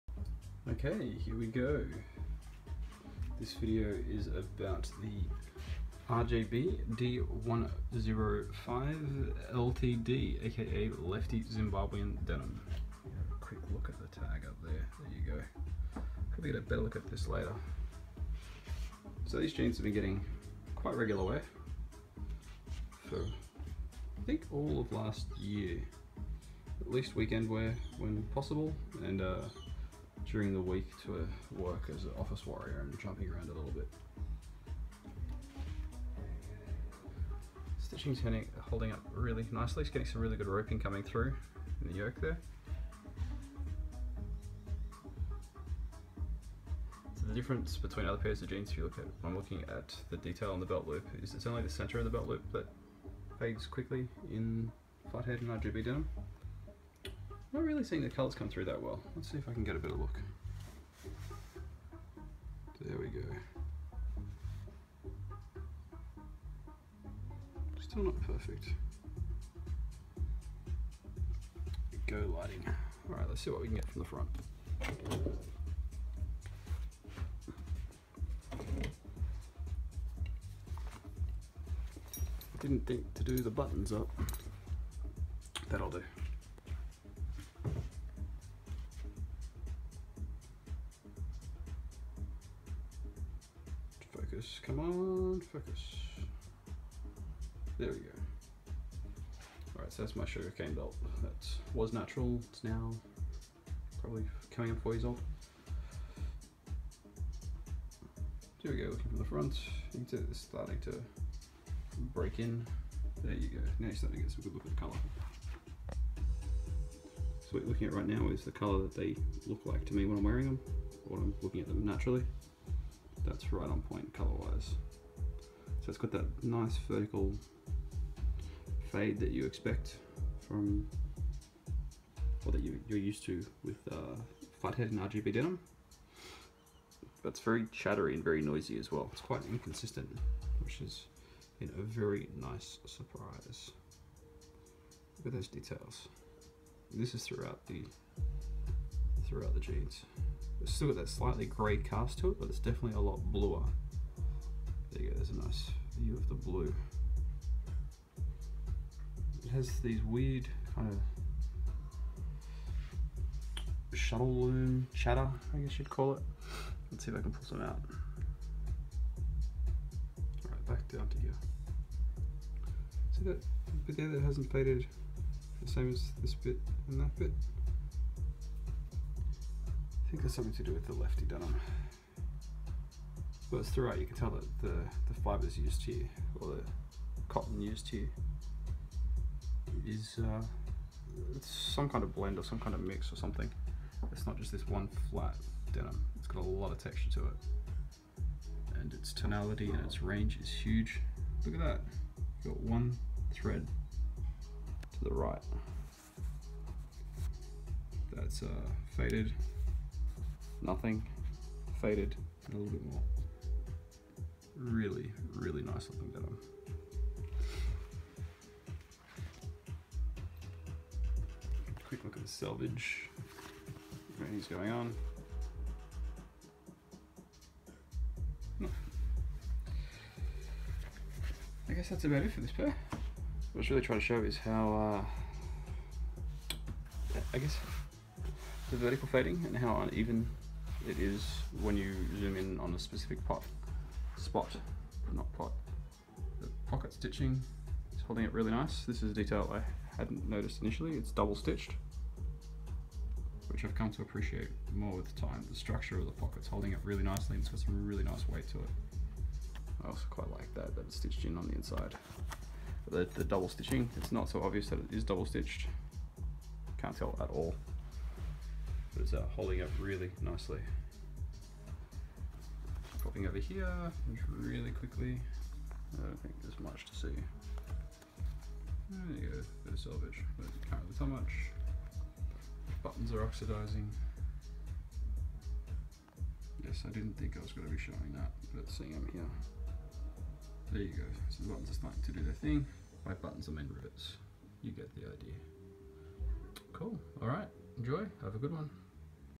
RJB LTD 105D Review
These have had a fair bit of wear over the past year. The character of the denim really starting to come through. Let's take a look!